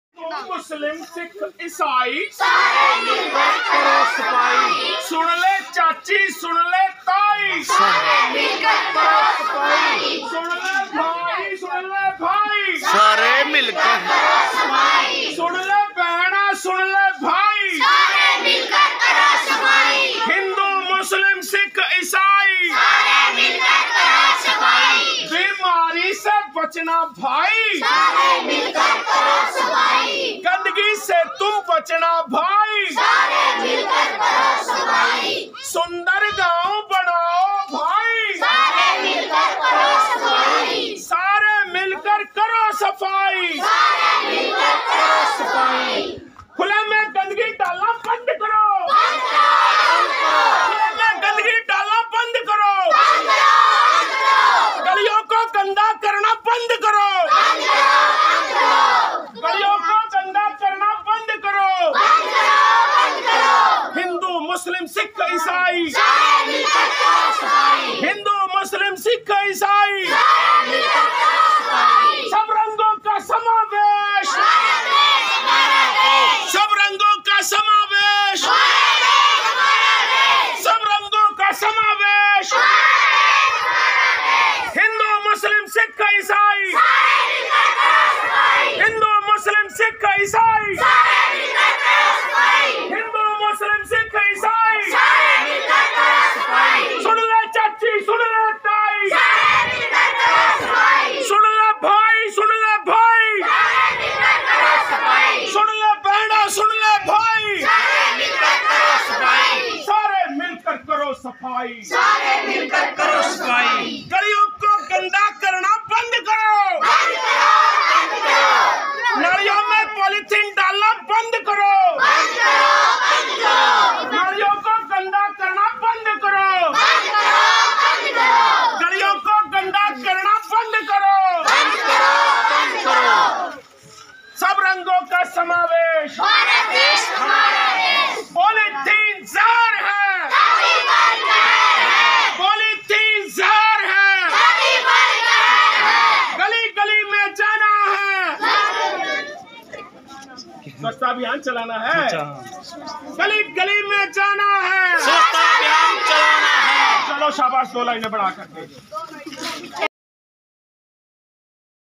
हिंदू मुस्लिम सिख इसाई, सारे मिलकर करो सफाई। सद्भाव, एकता और स्वच्छता का संदेश। Arun Kaharba
सब रंगों का समावेश, भारत देश हमारा देश।
उत्तर प्रदेश की सीमा से सटे करनाल जिला के गांव गढ़पुर टापू में राजकीय प्राथमिक पाठशाला के विद्यार्थियों व ग्रामीणों के साथ मिलकर सद्भाव, एकता और स्वच्छता का संदेश देने का एक प्रयास।